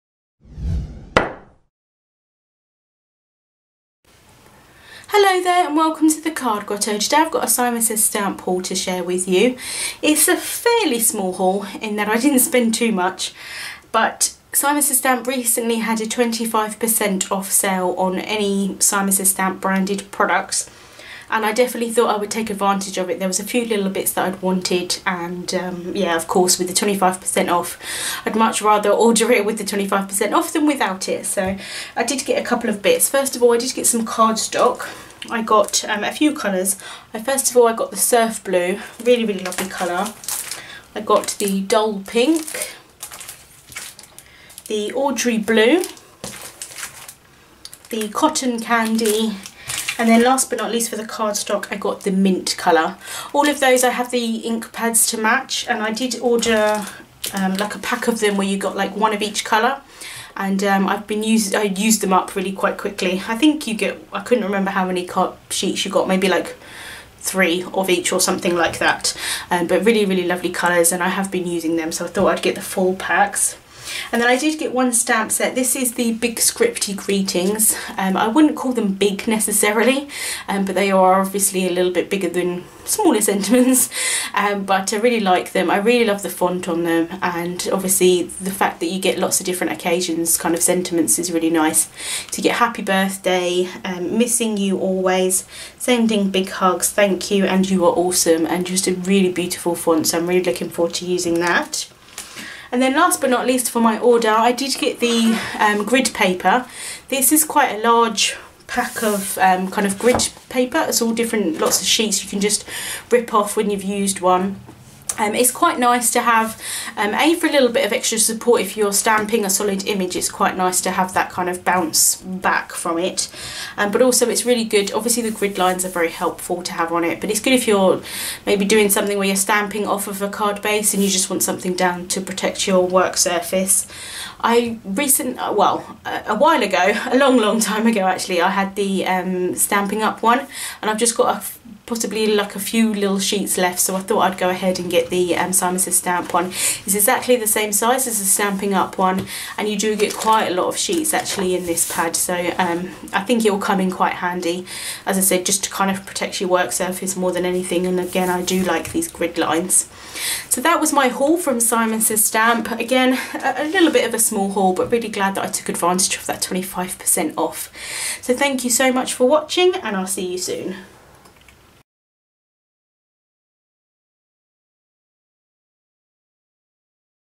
Simon Says Stamp Haul #10 | The Card Grotto
Hello there, I am back today with a small haul from Simon Says Stamp. I hope you enjoy watching. Links to the products are below.

——— PRODUCTS ———